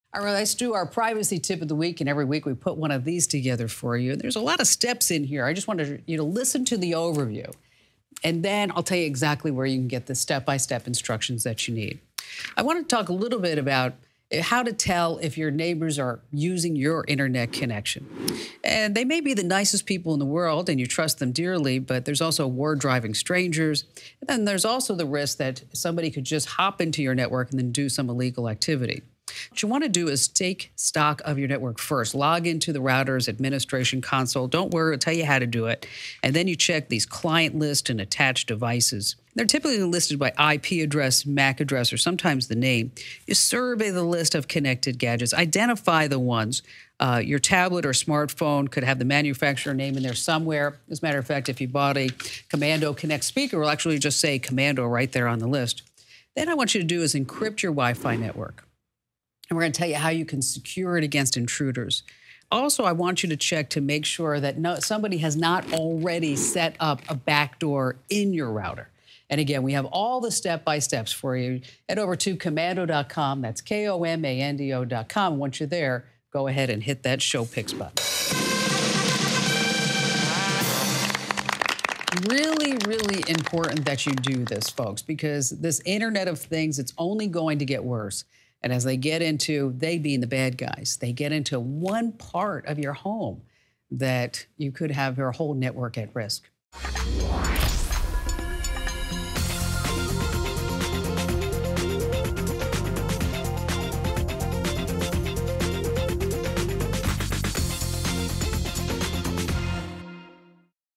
How you can tell if your neighbors are using your internet connection
Keeping your home Wi-Fi network secure is key to keeping out hackers and thieves trying to steal your information. In this Privacy & Security Tip, Kim Komando tells you how to secure your network and keep your information and family safe.